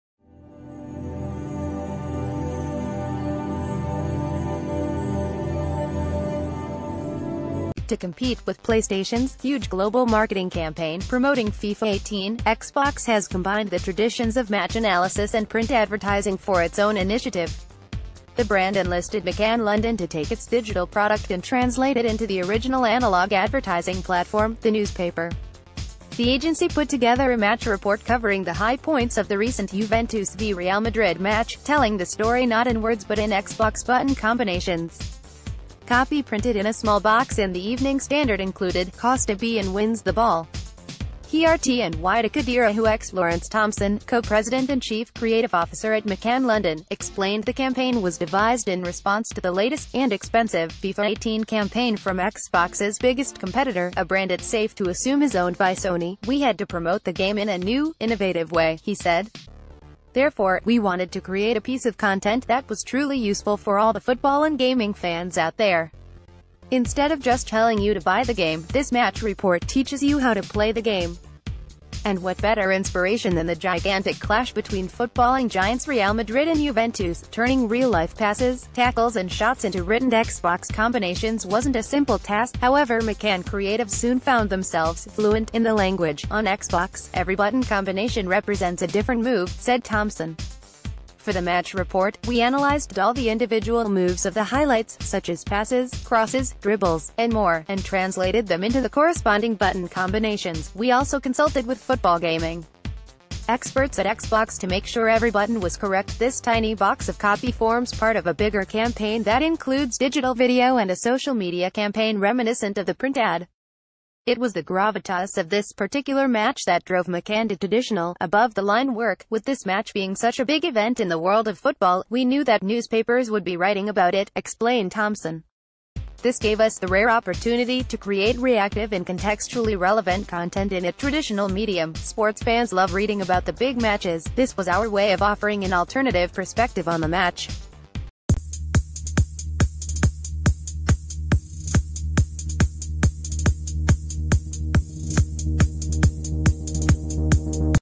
Breaking News | How Xbox translated Fifa controller inputs into a newspaper match report ad
To compete with PlayStation’s “huge global marketing campaign” promoting Fifa 18, Xbox has combined the traditions of match analysis and print advertising for its own initiative. The brand enlisted McCann London to take its digital product and translate it into the original analog advertising platform – the newspaper. The agency put together a match report covering the high points of the recent Juventus v Real Madrid match, telling the story not in words but in Xbox button combinations. Copy pri...